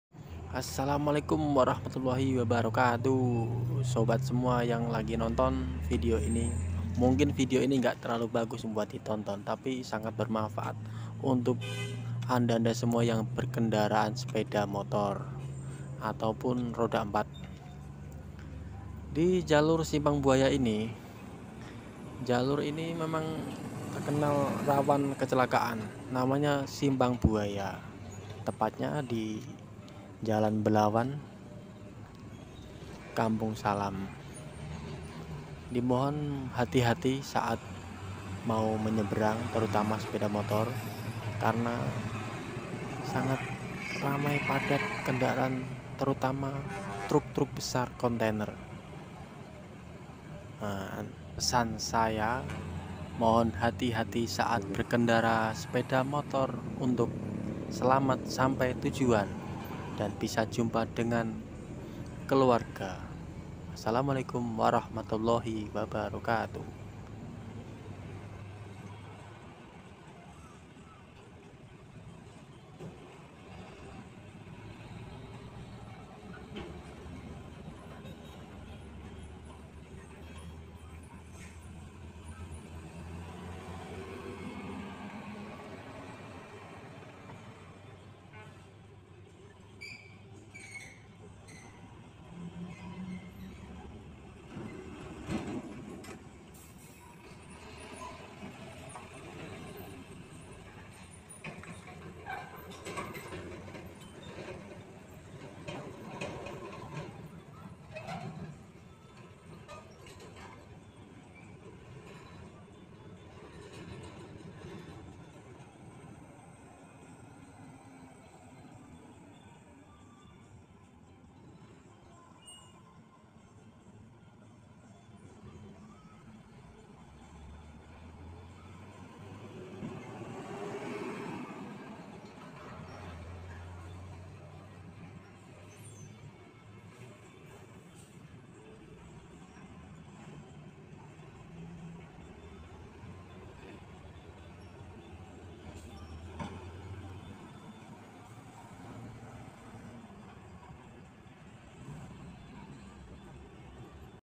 ramai jalan simpang buaya
jalan simpang buaya adalah simpang 3 di kampung salam.
simpang ini lumayan padat terutama truk kontainer truk truk besar yang melewati nya. karna jalan ini ada akses jalan ke arah tol Belmera. jadi otomatis ramai jalan ini di lewati mobil truk dan truk kontainer.